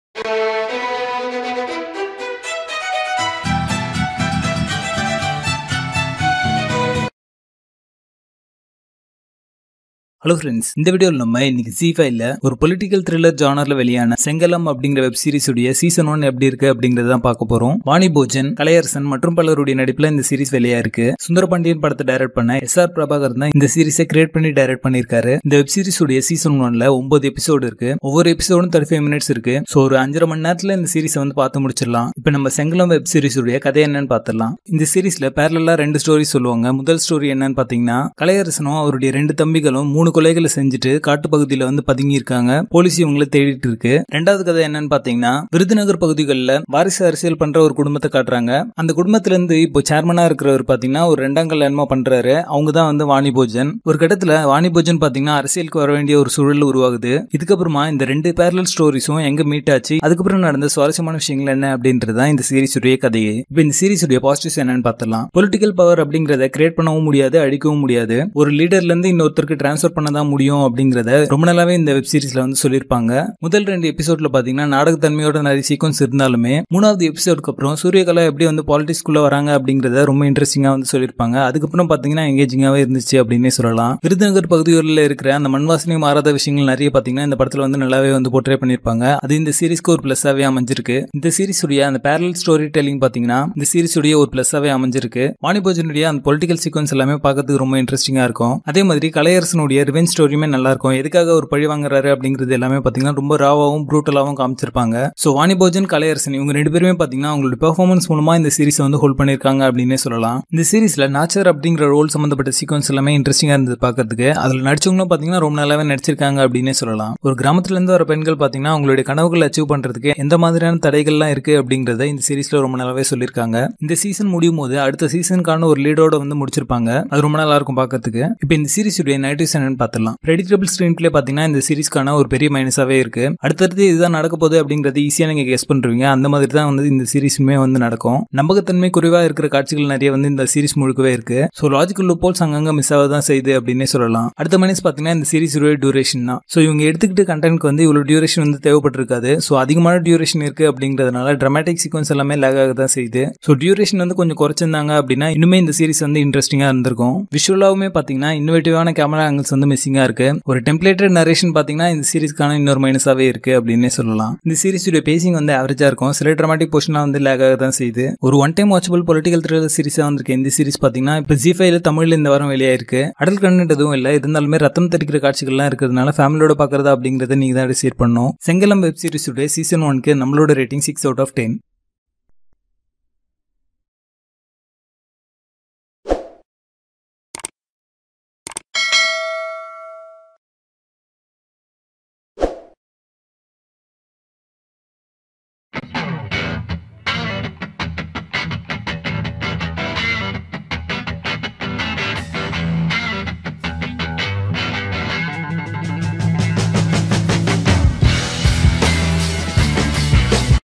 Sengalam (2023) Movie Review Tamil
Sengalam
2020 | 1h 32m | Action
Director: S.R. Prabhakaran
Cast: Vani Bhojan, Kalaiyarasan
Parental guide: UA